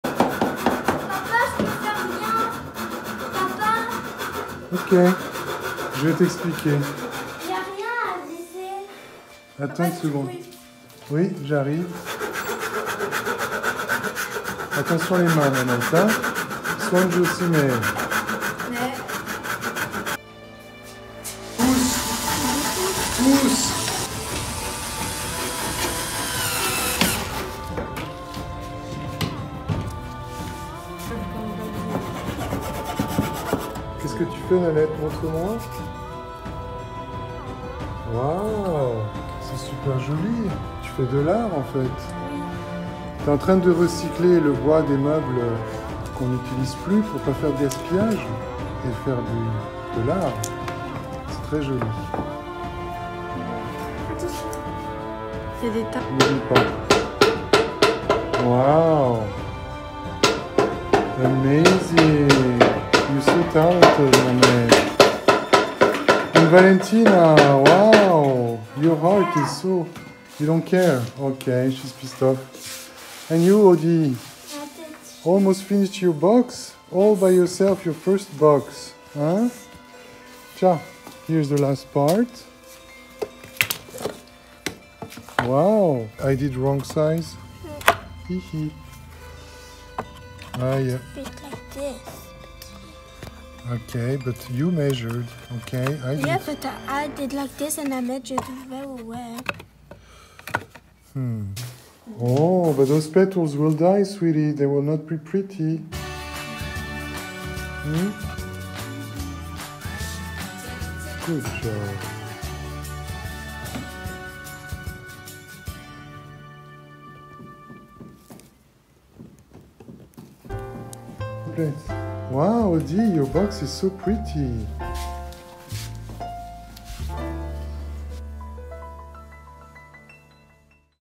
(프랑스 일상) 망가진 서랍장에서 탄생하는 각자의 재활용 아이디어
You want to throw your old broken pieces, don't do! There's always a way to recycle